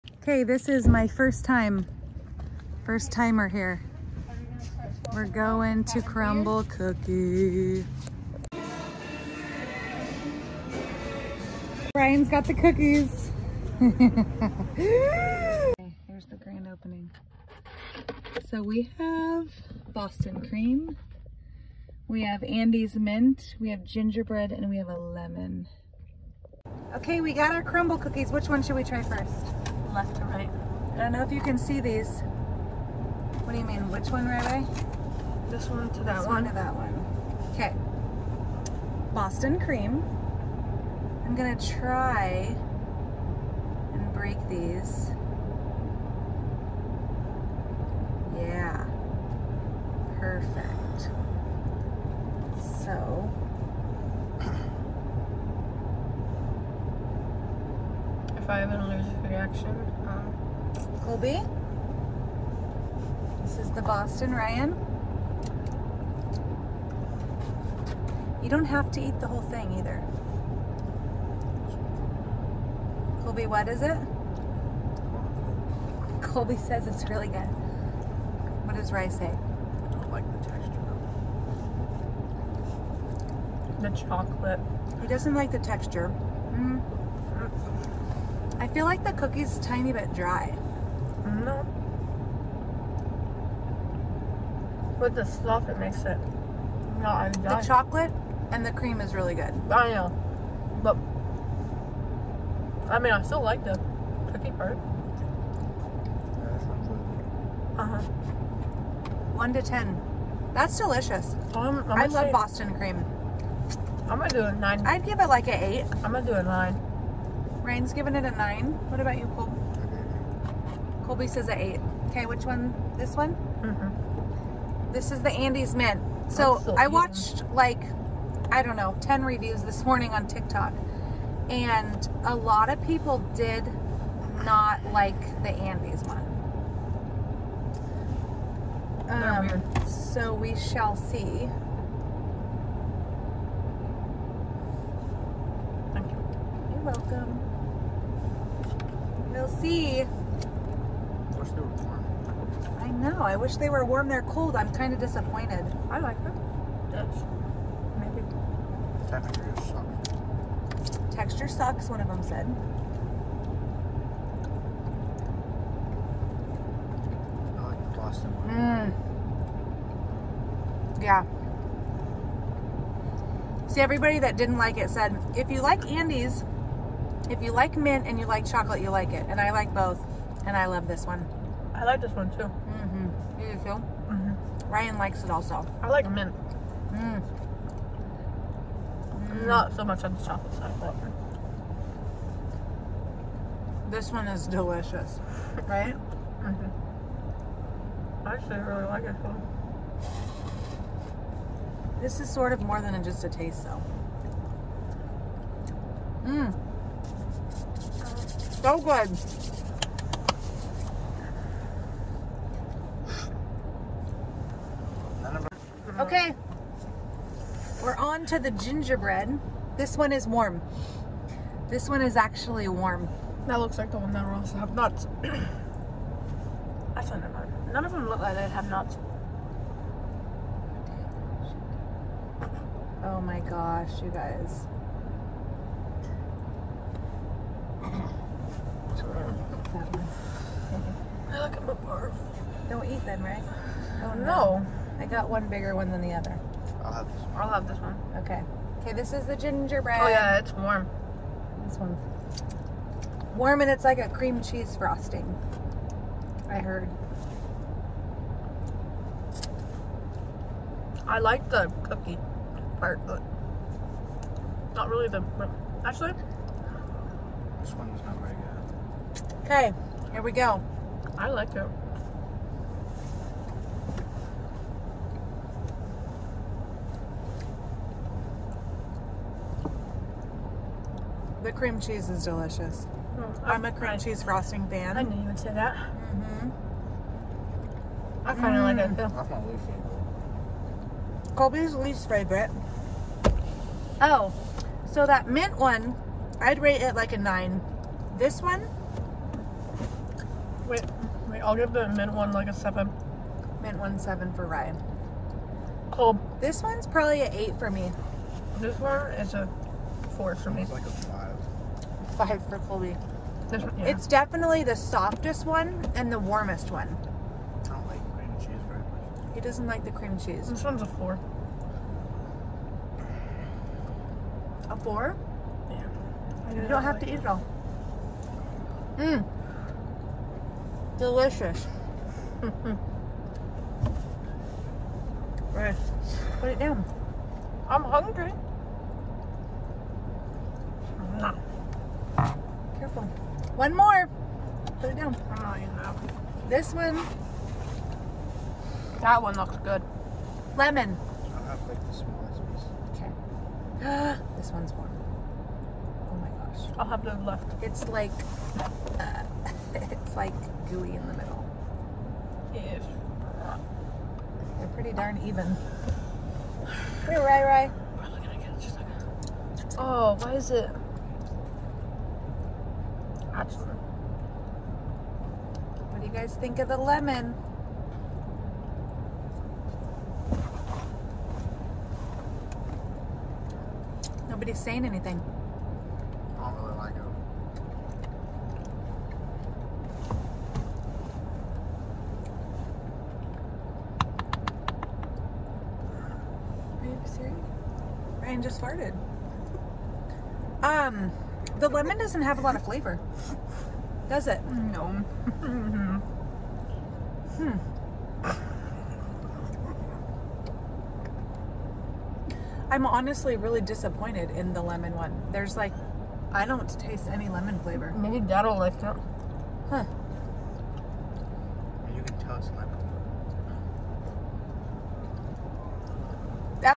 Crumbl cookie review not edited. First timers, reviewing and trying the cookies :)
If you make it toward the end it gets a little funny for a second. Boy mom ;) Also my son in the back wasn't feeling well that day.

This weeks flavors:

Gingerbread Cake 8/10
Lemon Crinkle 5/10
Green mint ft. Andes 9/10
Boston Cream Pie 8/10